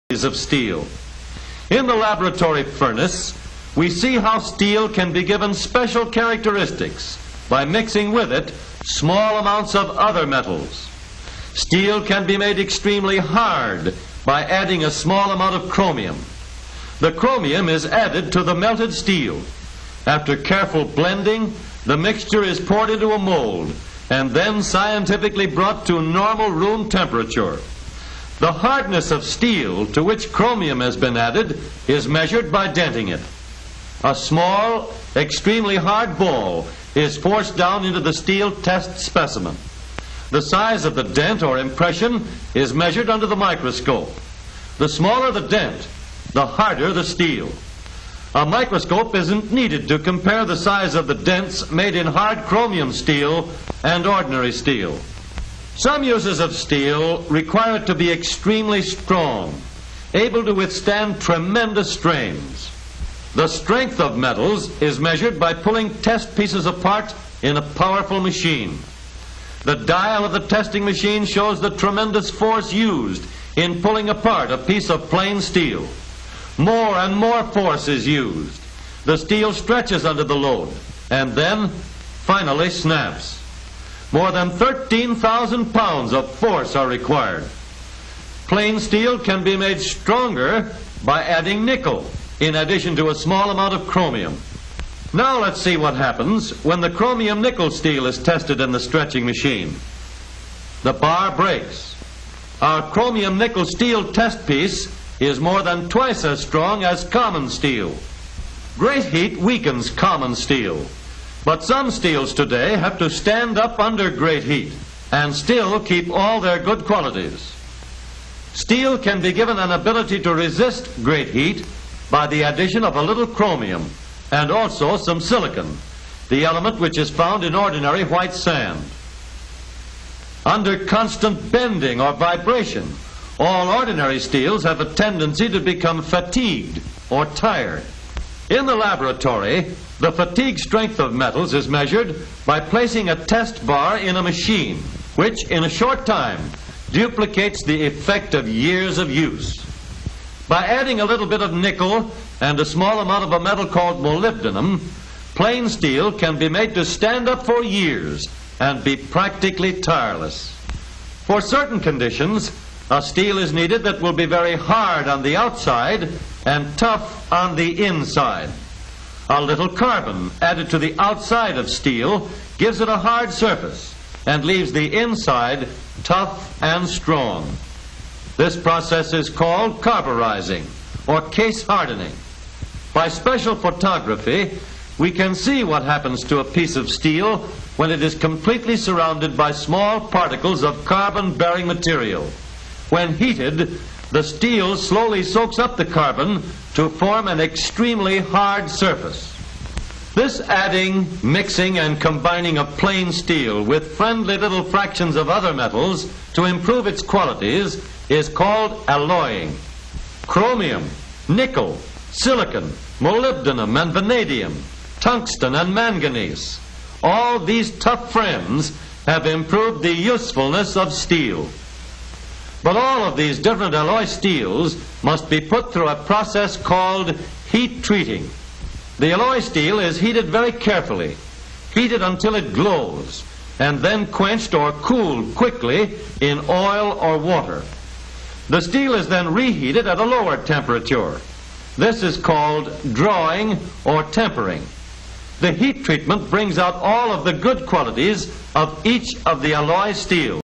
Hợp kim hoá và xử lí nhiệt trong luyện thép (Steel alloying & heat treatment)
Đoạn video này trích từ video về vật liệu cho ngành công nghiệp ô tô của hãng Chevrolet thực hiện.
Về nguyên tắc, các chi tiết khác nhau sẽ dùng loại thép với đặc tính yêu cầu khác nhau, để làm điều đó thì phải biết về alloying và heat treatment.
Dịch và làm phụ đề: Lâm Viết Hoan